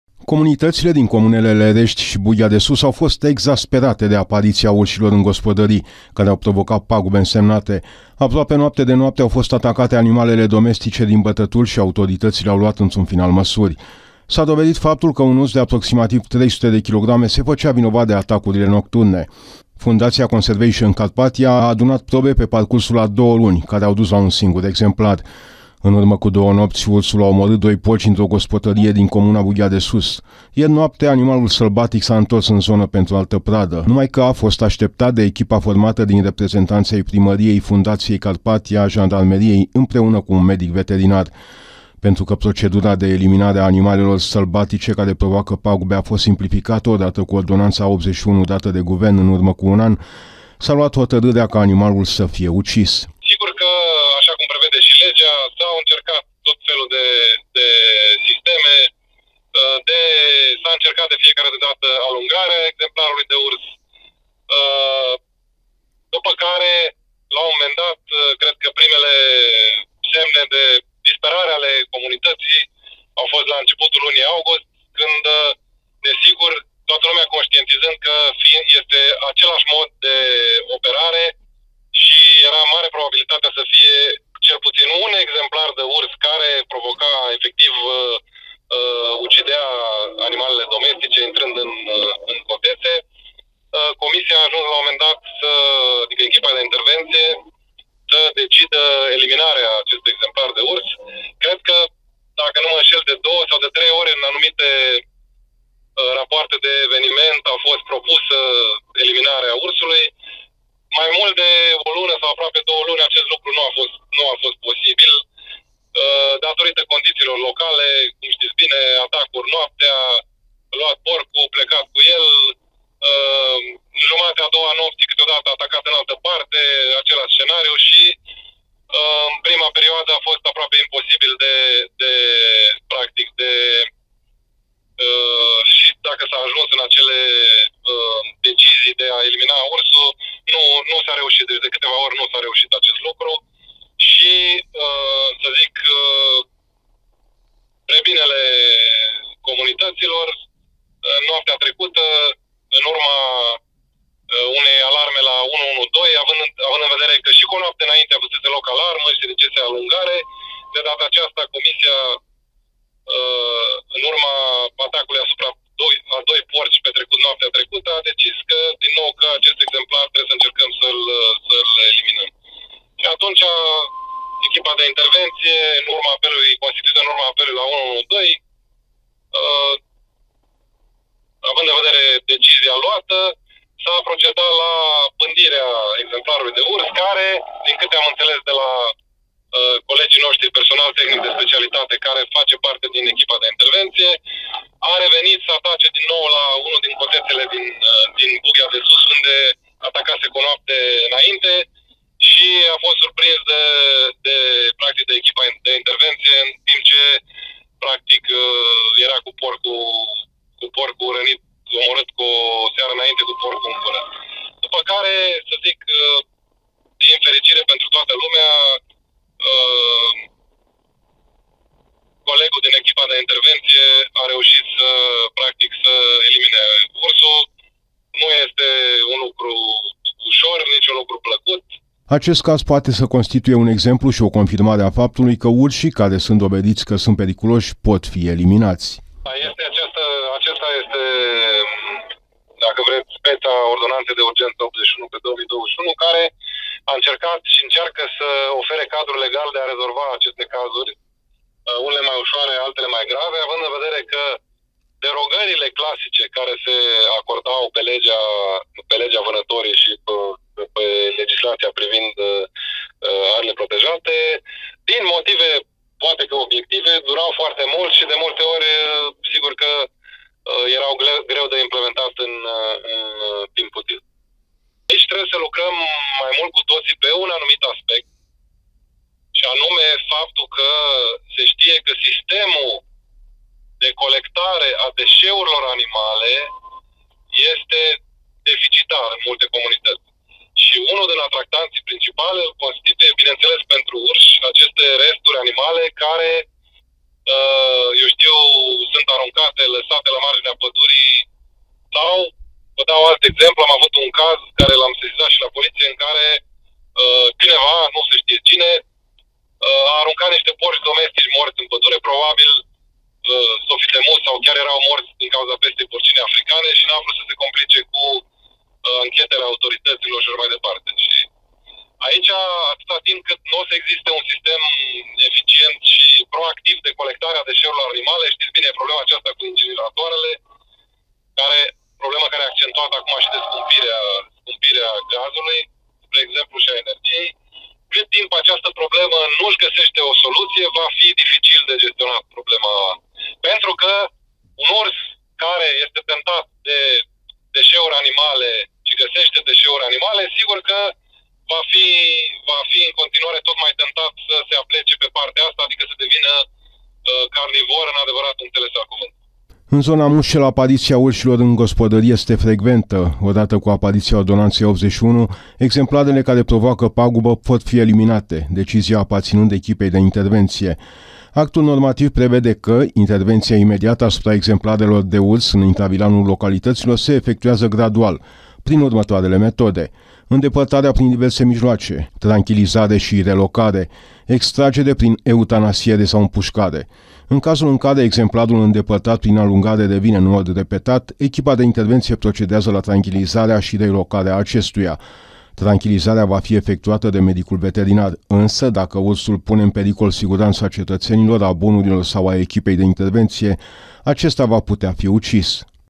Jurnal MUSCEL TV 31.08.2022 Urs împușcat la Bughea de Sus
Ursul care a omorât aproape 20 de animale în ultimele două luni în comunele Lerești și Bughea de Sus a fost împușcat noaptea trecută. Autoritățile au luat probe și sunt sigure că este vorba despre același urs care a făcut pagubele în gospodăriile muscelenilor.